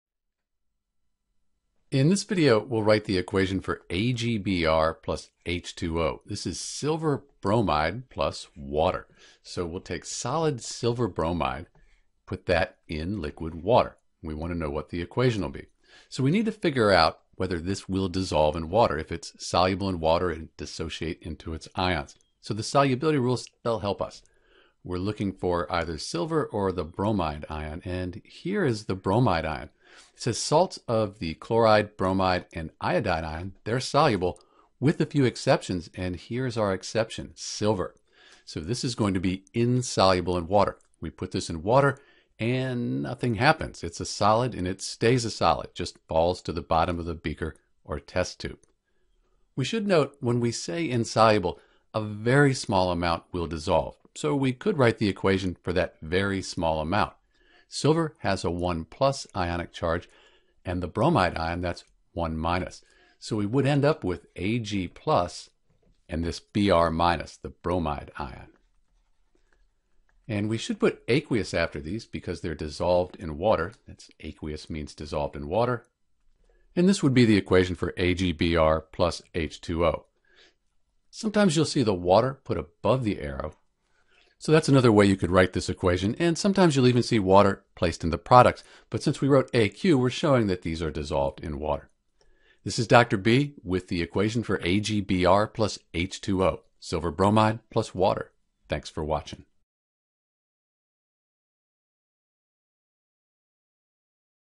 How to write the equation for AgBr + H2O (Silver bromide + Water)
In this video we will describe the equation AgBr + H2O and write what happens when AgBr is dissolved in water.

We can consult a solubility table to check and determine if AgBr will dissolve in water. There are also a set of general rules to help remember which compounds are soluble, insoluble, or partially soluble.

When we check the solubility table, we see that AgBr is insoluble. Essentially you end up with wet AgBr when you mix AgBr with water. Silver bromide doesn’t dissolve or react with the water. Note that a miniscule amount may dissolve but this is small enough that we can ignore it and say that AgBr is insoluble in water.

Knowing the solubility rules can help you determine whether AgBr will dissolve in water and dissociate into ions